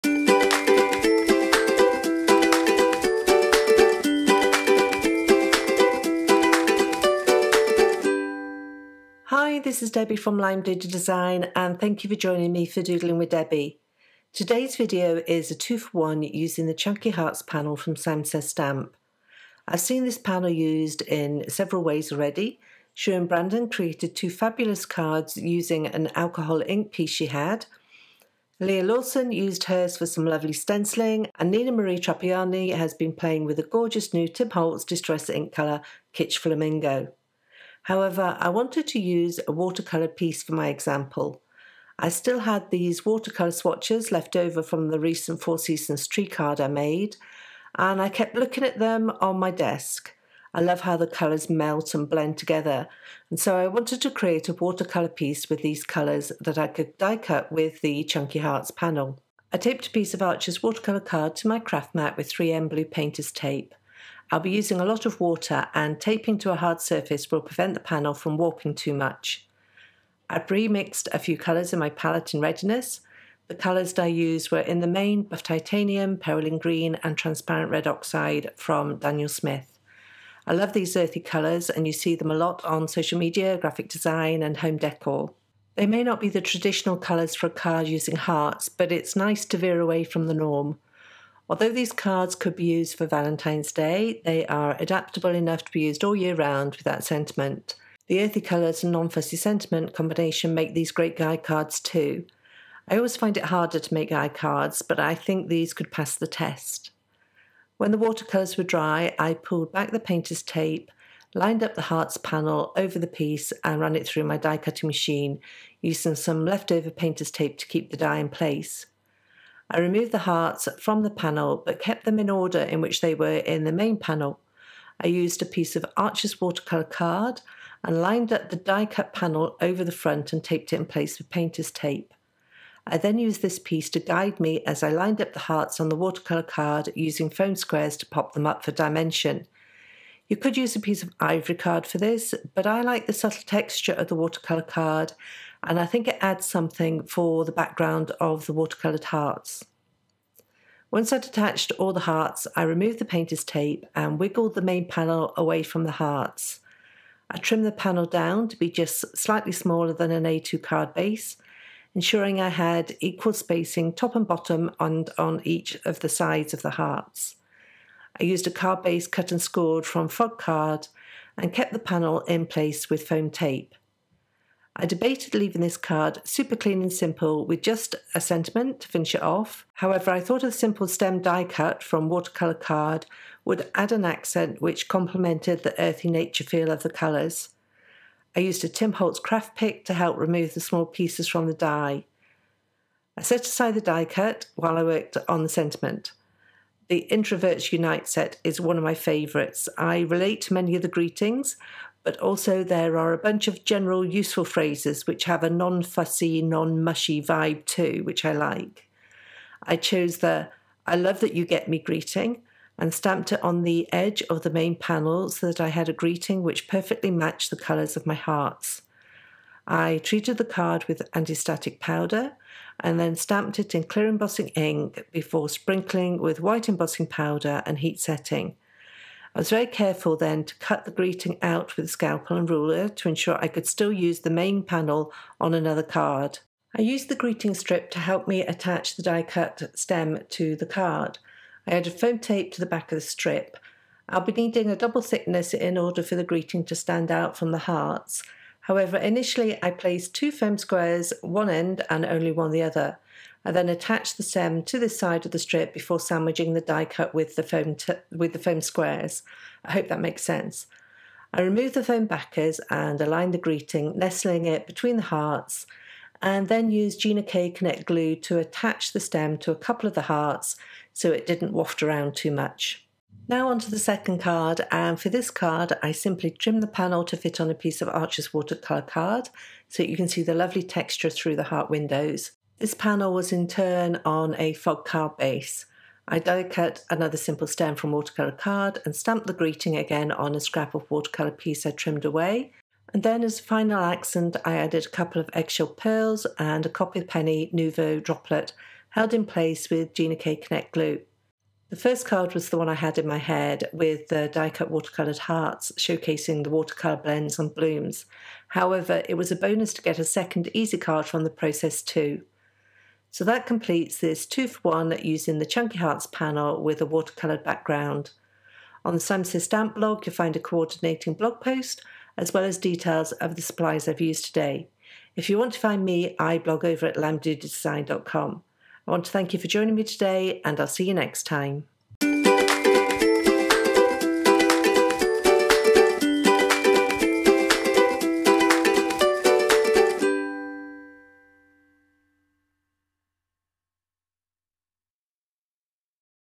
Doodling with Debby: Two for one Valentine's Cards
Follow along as special guest Debby Hughes shares two fun cards in about the time to make one on the latest edition of Doodling with Debby: our monthly blog series!

• Simon Says Stamp CHUNKY HEARTS PANEL Wafer Die sssd112288 Diecember

• Simon Says Stamp SIMPLE STEM Wafer Die sssd112269 Diecember

• CZ Design Stamps and Dies INTROVERTS UNITE set379iu Love You

• CZ Design Clear Stamps INTROVERTS UNITE cz364c Love You

• daniel smith at Simon Says STAMP!

• Little Things From Lucy's Cards Pearls GLOSSY EGGSHELL LBP02

• Crafter's Companion SOFTGRIP STRAIGHT BLADE CRAFT KNIFE Tool With Spares cc-tool-knife-st

• 3M Scotch SAFE RELEASE MASKING PAINTER'S TAPE Blue 03680

• Scrapbook Adhesives 3D 217 WHITE FOAM Squares Adhesive 01614

• MISTI MEMORY MISTI PRECISION STAMPER 12.25x12.25 Stamping Tool Kit mistimem

• MINI MISTI PRECISION STAMPER VERSION 2.0 Stamping Tool Kit mistimini

• Simon Says Stamp SMALL TEFLON BONE FOLDER Tool SSSTF

• Tim Holtz Tonic MAXI GUILLOTINE COMFORT PAPER TRIMMER 1980e